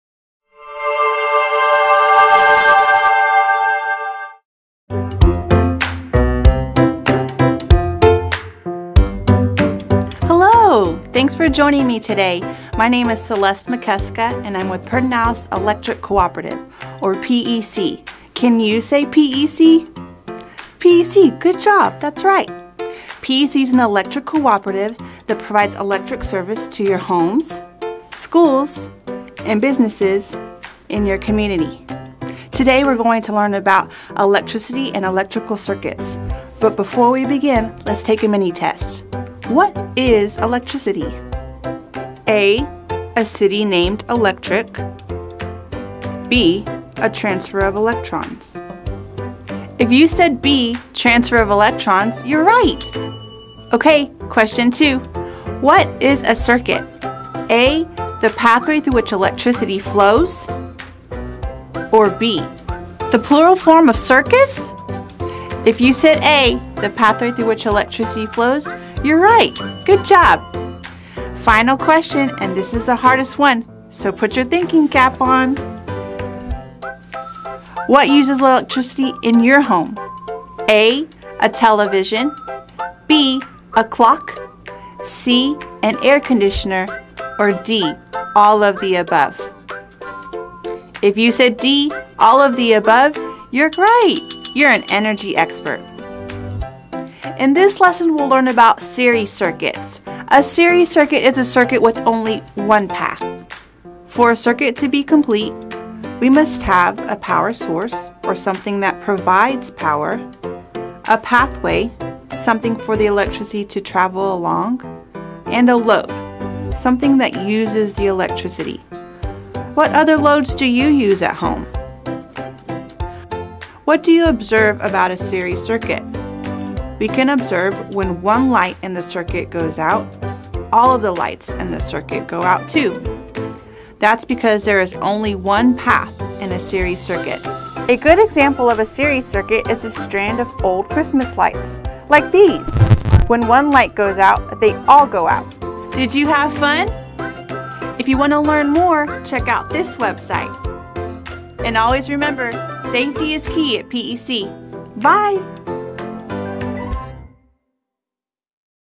Say Watt? Part 1: Series Circuits
Our Circuit Builder series teaches young learners all about electricity and electrical circuits. Students will learn about different types of circuits and how they work. In this introductory video, you’ll learn what a circuit is and be introduced to a specific type of circuit called a series circuit.